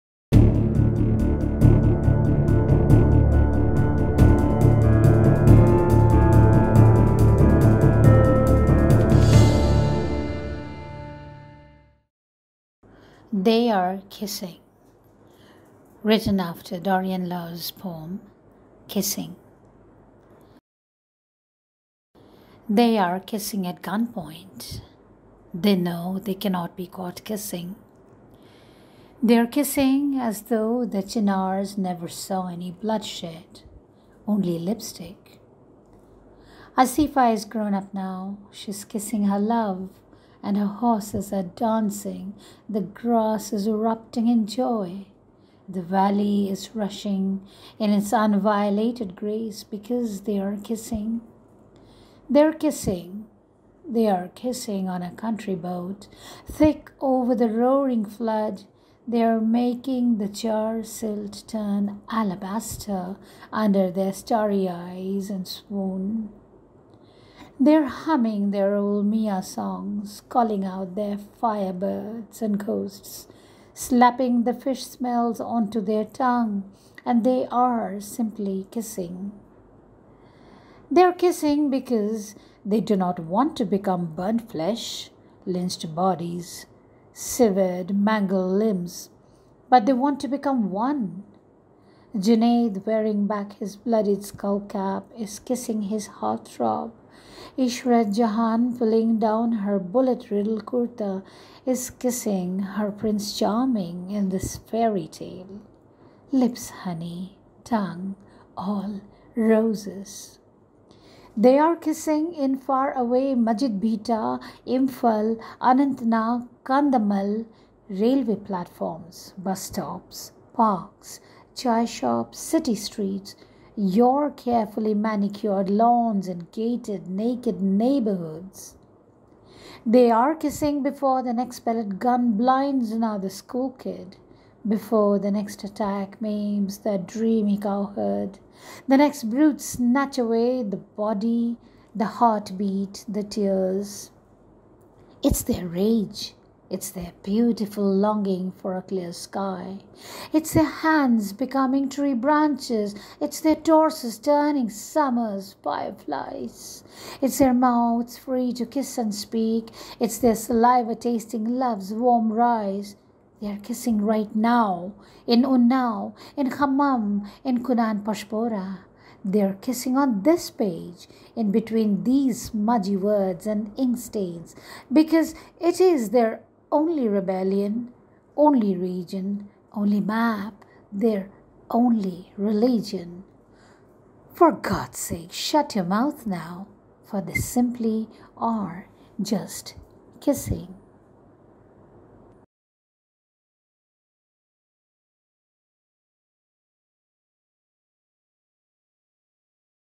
They are Kissing | Nabina Das reads her poem
Every day we see new evidence of authoritarianism in India. We are turning into a country in which citizens are murdered or attacked for being rational, for being critical, for raising voices of dissent, for just being themselves; for being Muslim or dalit or women. They want to tell us all what to eat, wear, read, speak, pray, think. They want to tell us how to live.

In these trying times, the act of love is a rebellion.

With paintings by Rollie Mukherjee, this week for Bol, poet and writer Nabina Das, through her poem “They are Kissing” shows us a world where love prospers, where Asifa, Junaid, and the countless others who lost their lives to hatred are alive and in love.